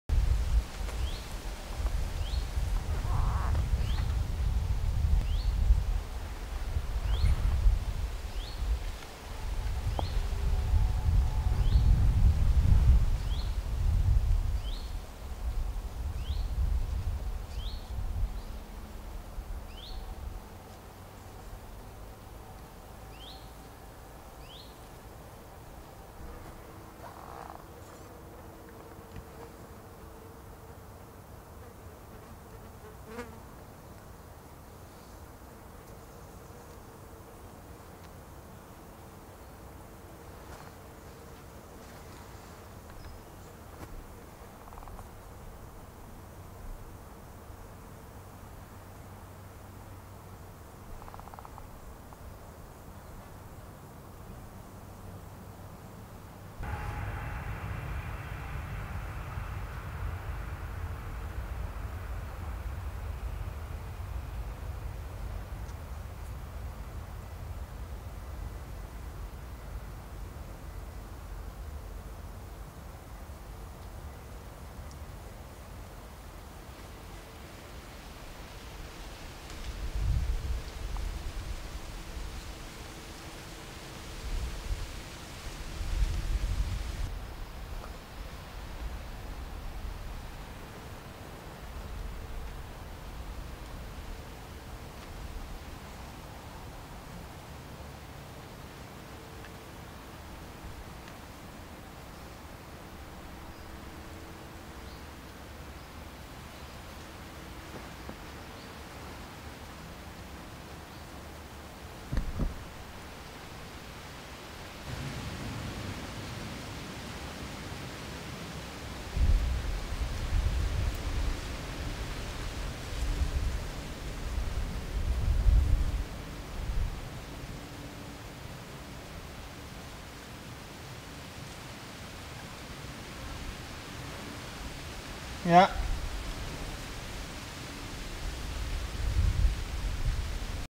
Vegetative knopige knotweed 🔴
Description (more): A number of vegetative plants of the knopige Millipede together. Here and there is a thriving to see copy with flower heads on a separate steel. The way in which the leaves on the stem are is clearly visible. Various frameworks. Also the leaf shape is well recognizable. Composite clip.